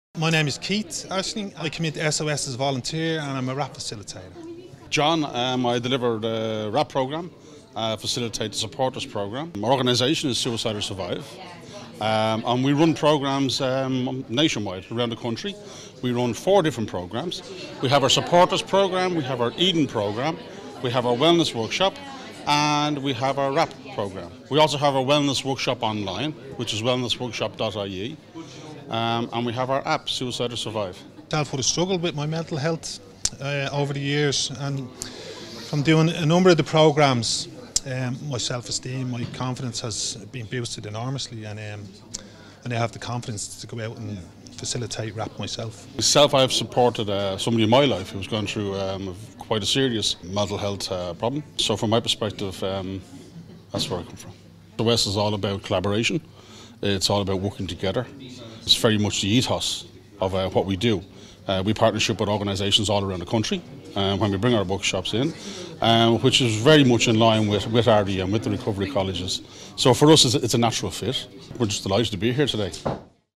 ARI Recovery Fair, May 2017: Suicide or Survive (SOS)
SOS (Suicide or Survive) at the ARI (Advancing Recovery in Ireland) Recovery Fair, Kilkenny, 18th May 2017.